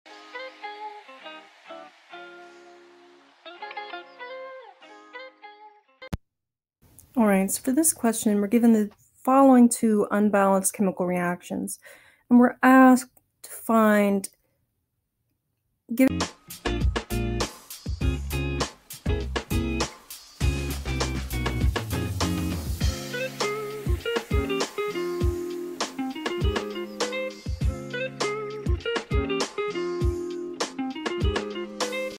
Methane (CH_4) reacts with oxygen in the air to produce carbon dioxide and wa…
Methane (CH_4) reacts with oxygen in the air to produce carbon dioxide and water. Ammonia (NH_3) reacts with oxygen in the air to produce nitrogen monoxide and water. What mass of ammonia is needed to react with excess oxygen to produce the same amount of water as 1.00 g of methane reacting with excess oxygen?

chemical equations simplified: learn the basics, mastering reaction stoichiometry: key concepts    amp; formulas, understanding the difference between mass and weight